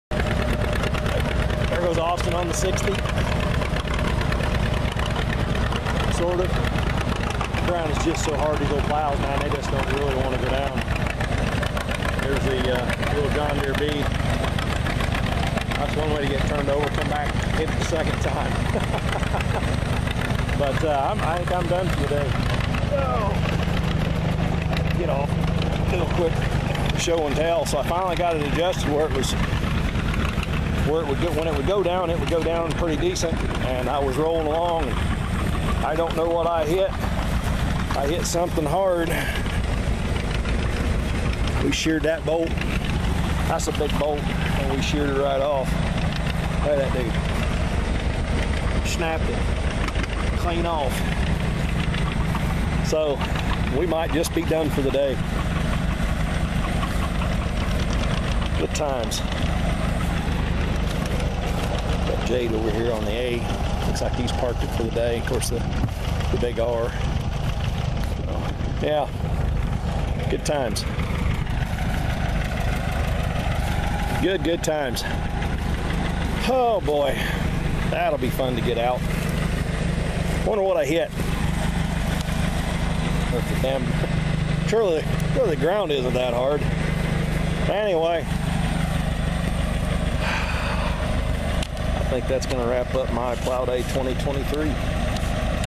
Plow Day 2023 part 3
a little plowing action and personal carnage. when a 3/4 inch bolt shears off you Might have to change your underwear...